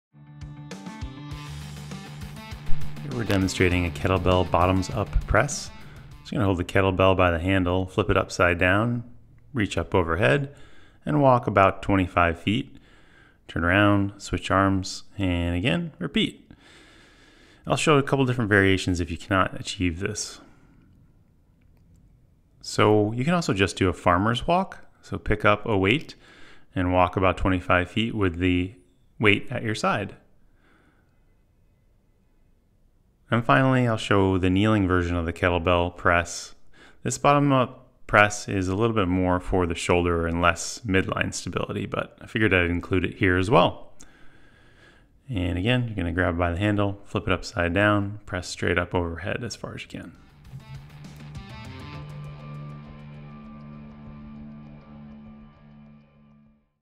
Home Exercises: Kettlebell Bottoms Up Press
Demonstrating the Walking Kettlebell Bottoms Up Press, the Farmer's Carry and the kneeling Bottoms Up Press. For more midline stability, use the first two. For more shoulder stability, use the last.

Check with your doctor for specific exercise prescriptions.